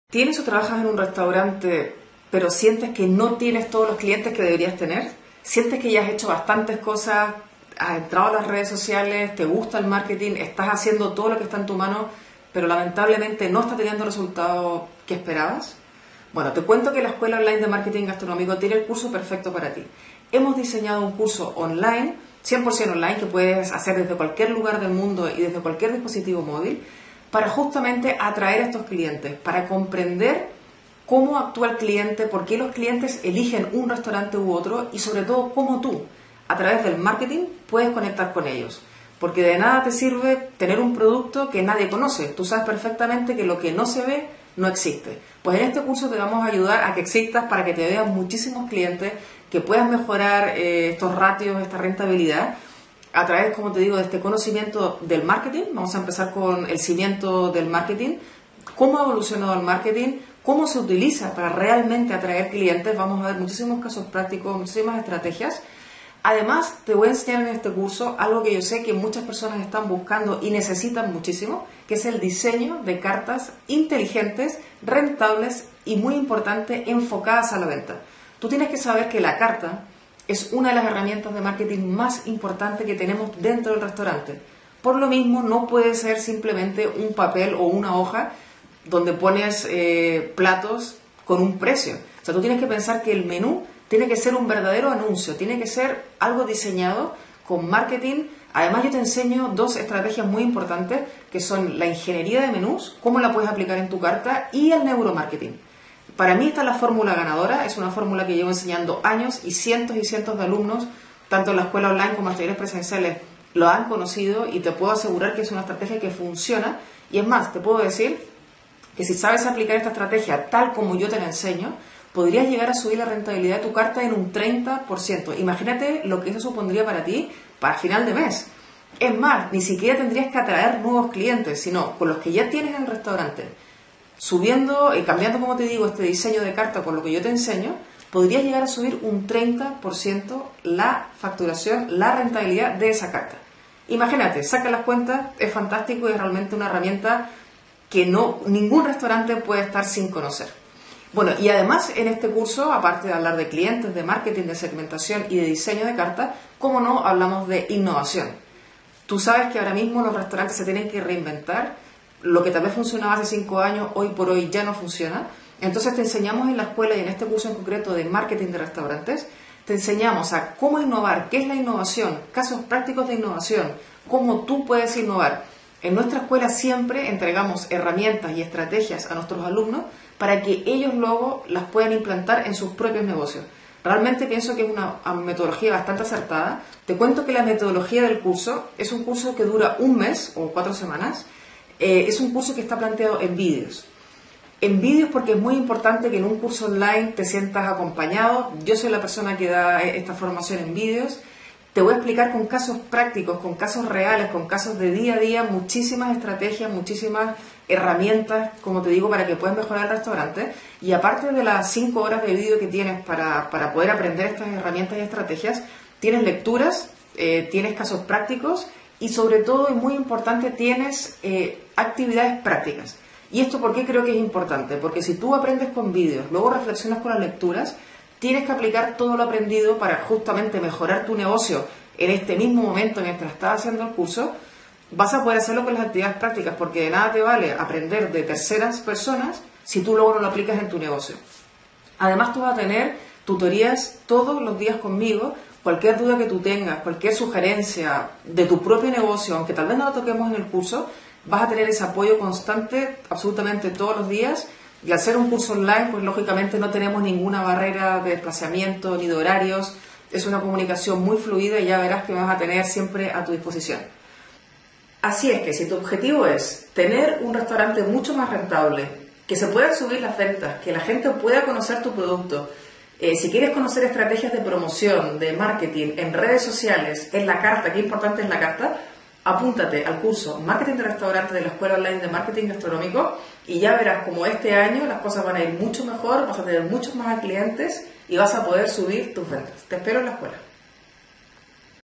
Diplomado en Marketing Gastronómico
Aprende en 3 meses los secretos del marketing gastronómico que aumentarán las ventas y la rentabilidad de tu negocio.

APRENDE A SER DIFERENTE, RELEVANTE Y MEMORABLE
EN UN MERCADO CADA VEZ MÁS SATURADO.

Descubre cómo este curso te ayudará a atraer más clientes en este enlace